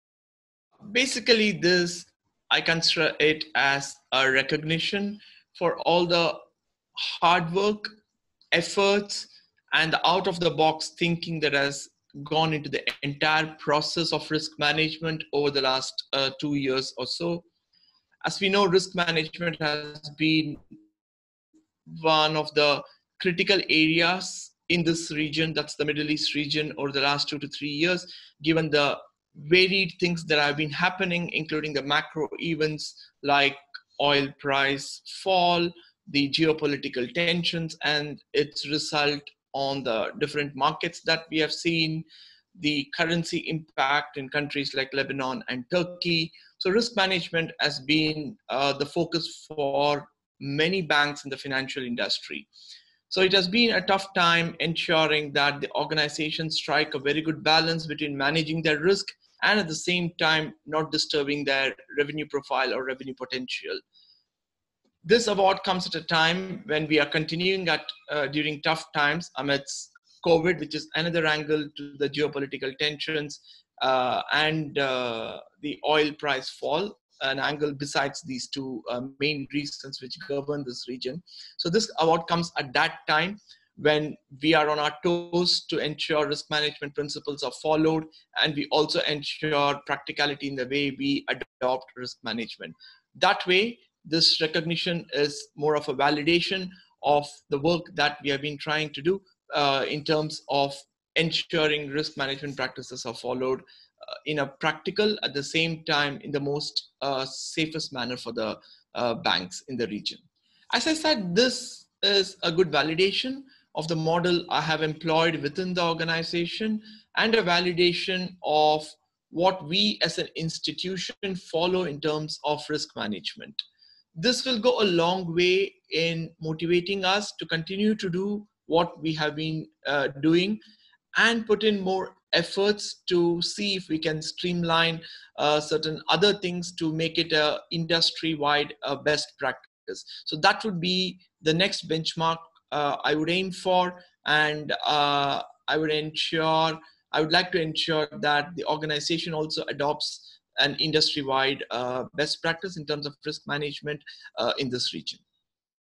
Deutsche Bank AG is a Stevie® Award Winner in The 2020 Middle East Stevie® Awards
The first edition of the Middle East Stevie® Awards, which recognize innovation in the workplace in 17 nations in the Middle East and North Africa, culminated with a virtual awards ceremony on August 25.